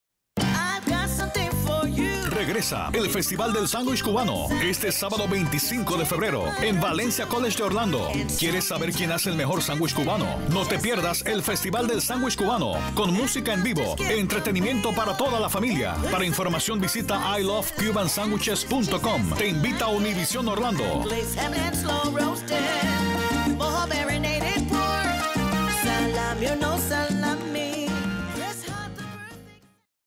Metro Orlando Cuban Sandwich Fest Univision Spot
Saturday, Feb 25th
Metro Orlando Cuban Sandwich Smackdown
@Valencia College
1800 S. Kirkman, Orlando Florida
Parking Lot C